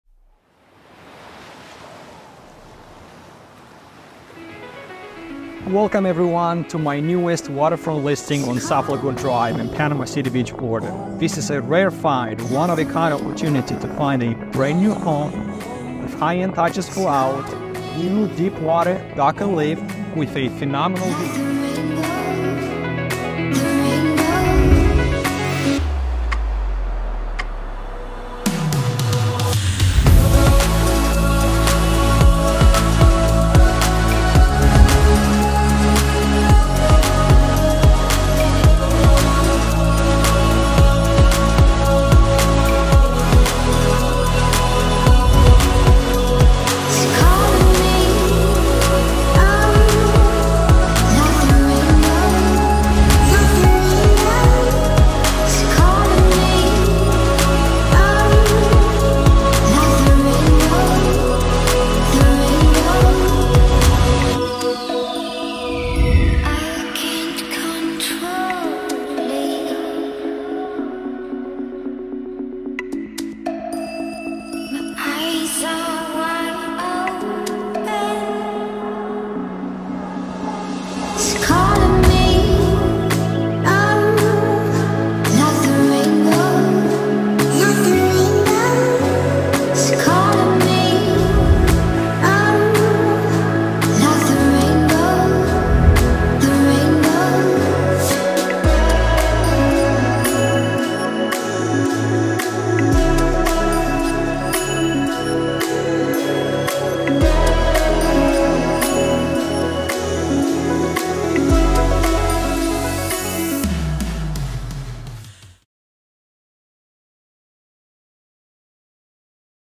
New Waterfront Home in Panama City Beach
WOW! Rare opportunity to own a luxurious, brand new waterfront home with a new 100' deep-water boat dock and lift. Nestled along the pristine shores of Panama City Beach, this brand new waterfront residence is a coastal dream come true! Offering the epitome of luxury coastal living, this home is a haven for those who crave stunning views, High End craftsmanship and the allure of waterfront living. Walking distance or a short golf cart ride to the Gulf of Mexico. STR allowed with amazing projected ROI. Step into a world of contemporary elegance with an open-concept design that seamlessly blends sophistication and comfort. The interiors are bathed in natural light, creating a warm and inviting atmosphere with stunning water views. Enjoy the luxury of having your private new (7'-9')deep water 100 foot boat dock with 14,000 lb lift for a 32' boat, a new seawall ensuring effortless access to the Gulf's azure waters. Whether it's a sunrise cruise or a fishing trip, the possibilities are endless. Wake up to panoramic views of the Lagoon waters. The expansive windows frame the picturesque scenes, creating a daily spectacle of natural beauty. Culinary enthusiasts will delight in the state-of-the-art kitchen featuring top-of-the-line SS appliances, sleek quartz countertops, custom maple kitchen cabinets, accent European style backsplash and shiplap overlooking the amazing views of the water. Unwind in the lavish master suite with custom pine ceiling beans, walk-in closet and a private access to the back deck overlooking the lagoon. The en-suite bathroom is a spa-like oasis, offering a walk-in shower, walk-in closet and a double vanity. Additional highlights include: Metal Roof, Spray Foam attic & exterior walls, 8 Foot Interior & Exterior Doors, Luxury Vinyl Plank Flooring throughout, New seawall, Garage plumbed for ice maker hookup, Gutters are attached to underground drain, Hardscape landscaping for little maintenance, Wired & plumbed to exterior walls for future outdoor kitchen, 500 gallon septic tank with drip irrigation system, 3 Ton HVAC unit with heat pump, Custom black out shades for living room and more. The water-front lifestyle extends to the outdoor spaces. Lounge on the spacious deck, host gatherings with friends and family, or simply bask in the coastal breeze. Ready to make water-front living your reality? Call to schedule a private tour and make this brand new coastal retreat your forever home